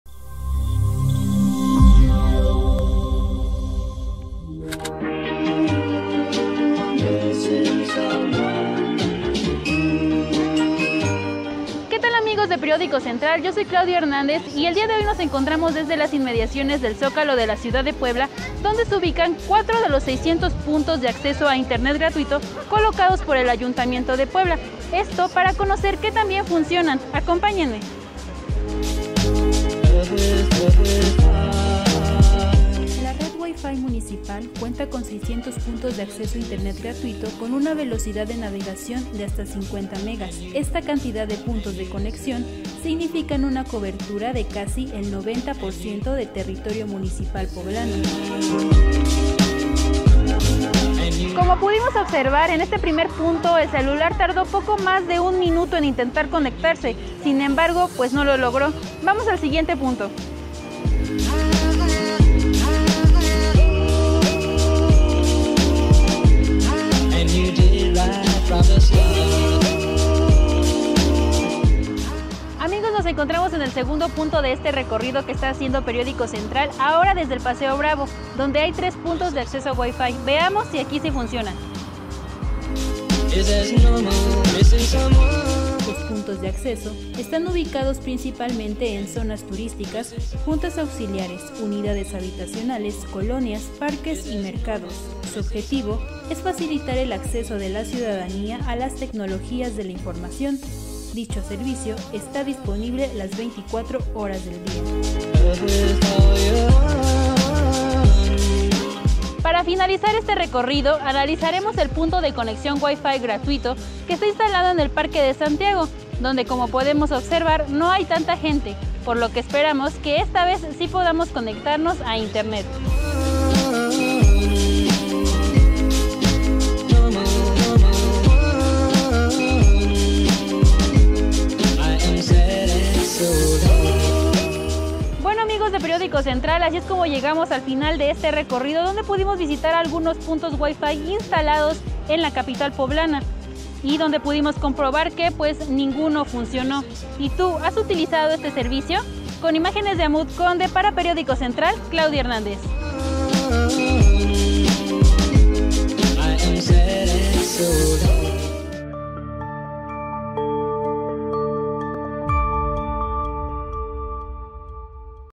Puntos Wifi gratuito el programa fallido de los gobiernos municipales de Puebla
Puntos Wifi gratuito, el programa fallido de los gobiernos municipales de Puebla. Desde el 2008 a la fecha, los ayuntamientos de la capital poblanas han visto como punto clave la ampliación y mejora de la red de internet sin costo

Desde 2008, la red de puntos de acceso a internet gratuito en Puebla capital ha incrementado hasta acumular un total de 600. Sin embargo, continúan sin funcionar al 100%.